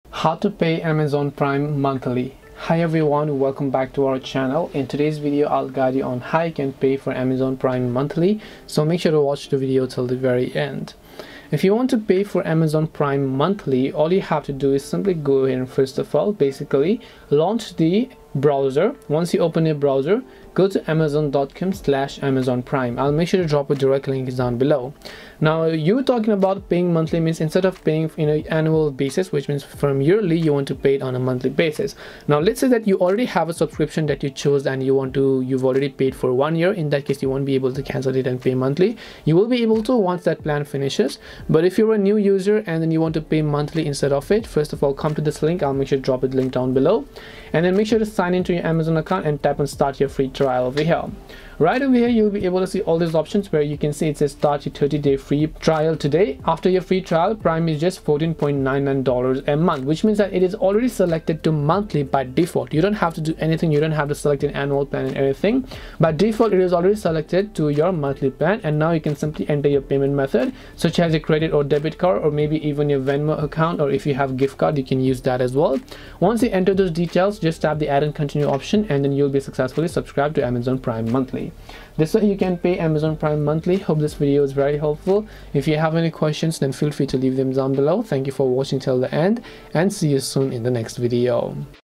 How to Pay Amazon Prime Monthly !
➜ This video guides you through an easy step-by-step process to pay for Amazon Prime monthly. So make sure to watch this video till the end.

~ Chapters:
0:00 Introduction
0:11 Pay Amazon Prime Monthly
1:22 Conclusion